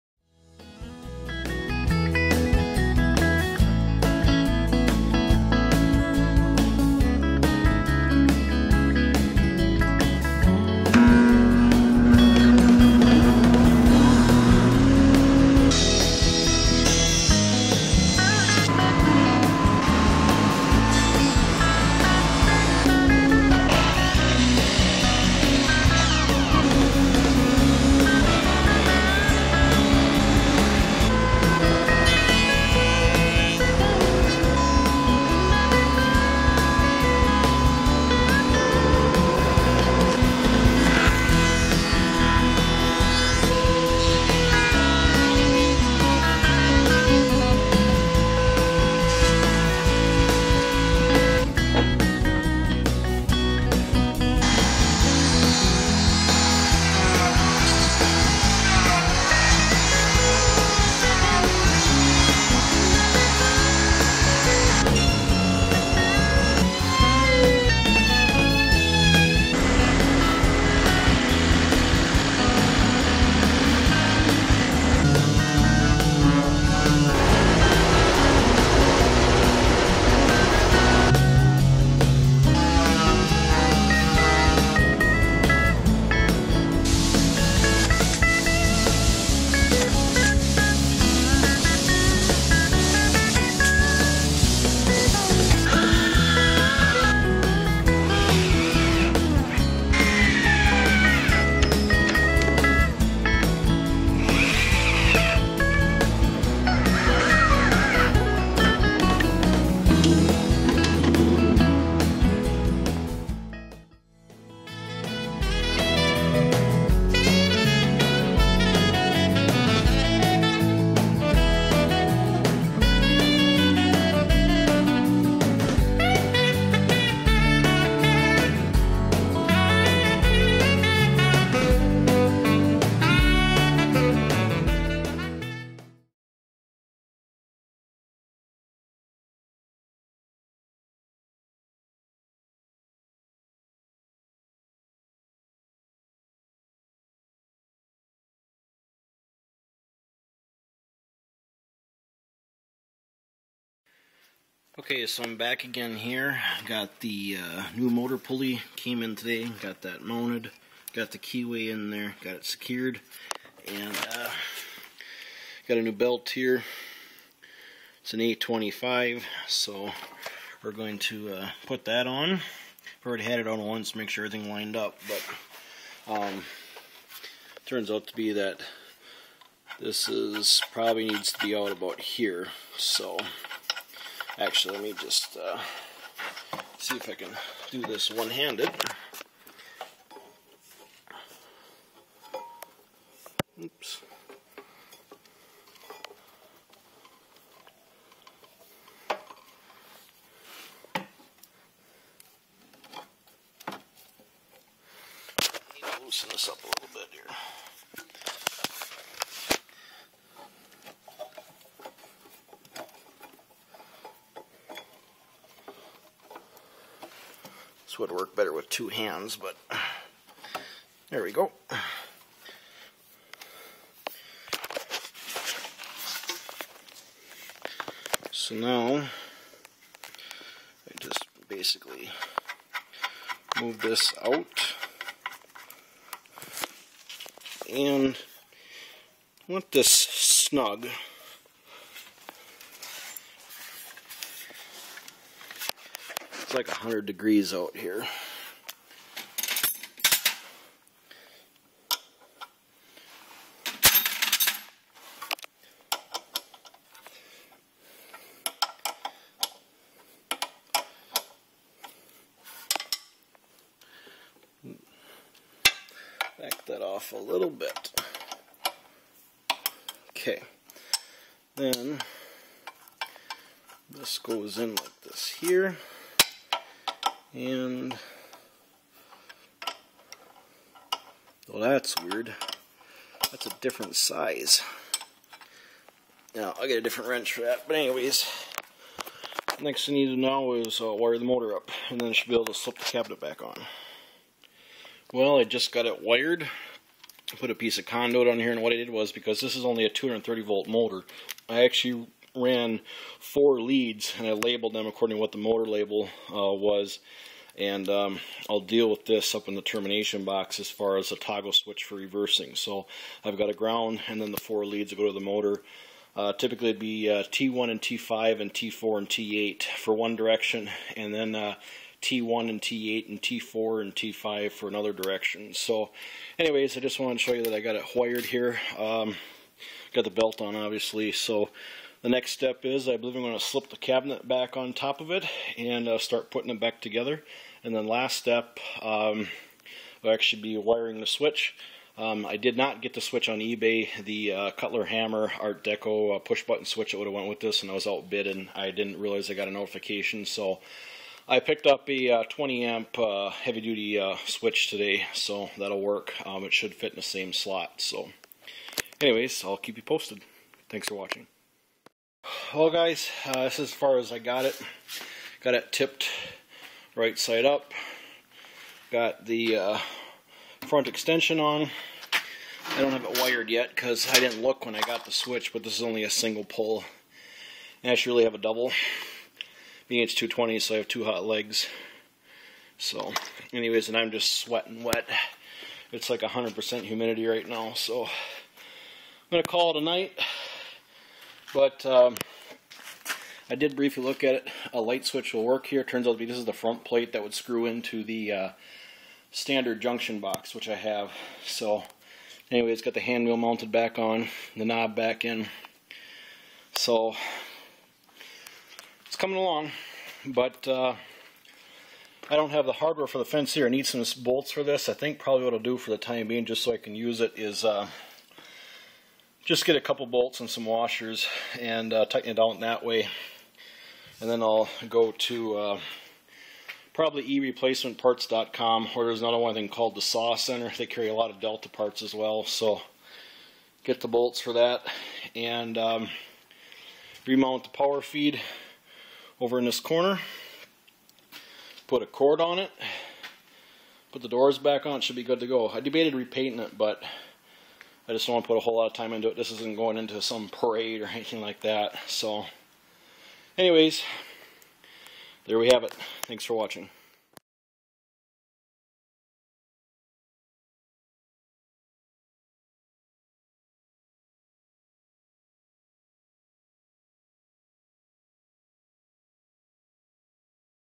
Delta Rockwell Heavy Duty Shaper part 2 # 76
A newly acquired Delta Rockwell heavy duty shaper
Overveiw of its current state and plans.

This unit was morethan likely converted to 3ph and I will be converting it back to single phase. I purchased a new Leeson 3HP 145T frame 3450rpm 230v 1ph motor to replace the 3HP NEMA 66 frame 3ph motor. I also am wating on a new 7" pulley from Graingers and an original Cutler-Hammer art deco red and black push button start-stop switch that would have been also original for this machine.